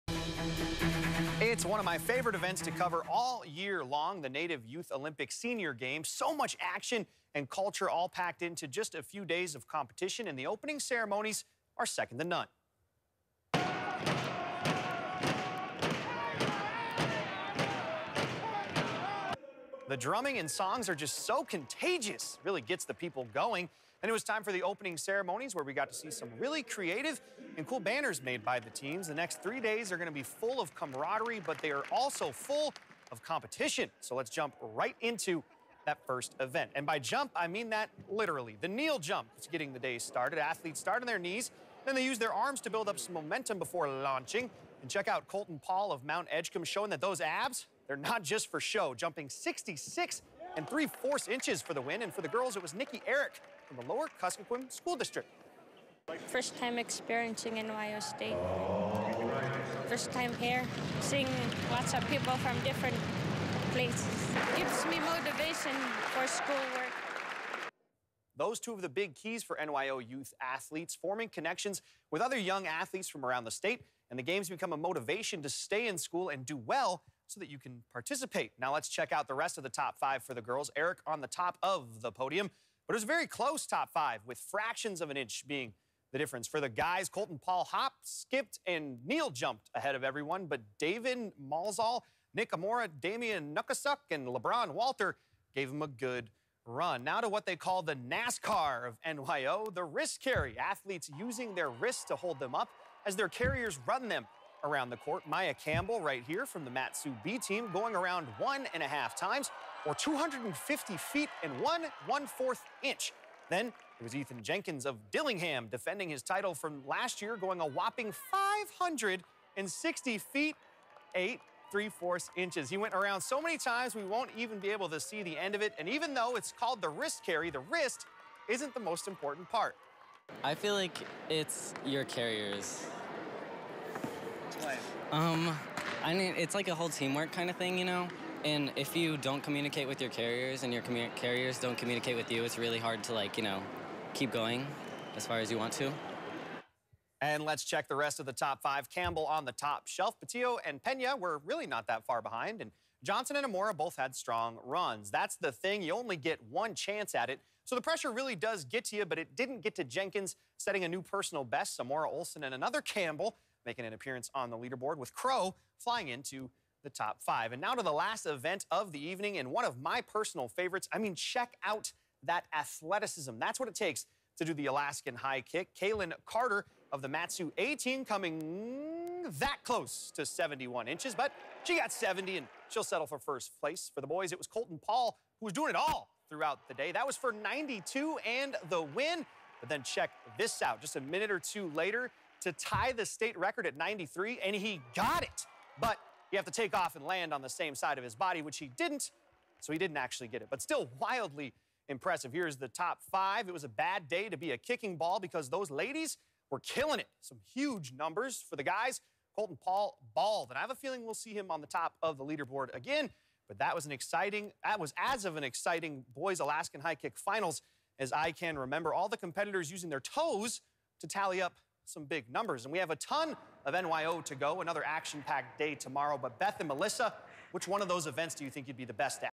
Native Youth Olympics Day 1
Opening ceremonies along with the Kneel Jump, the Wrist Carry and the Alaskan High Kick take center stage at the first day of the Native Youth Olympics Senior Games 2023.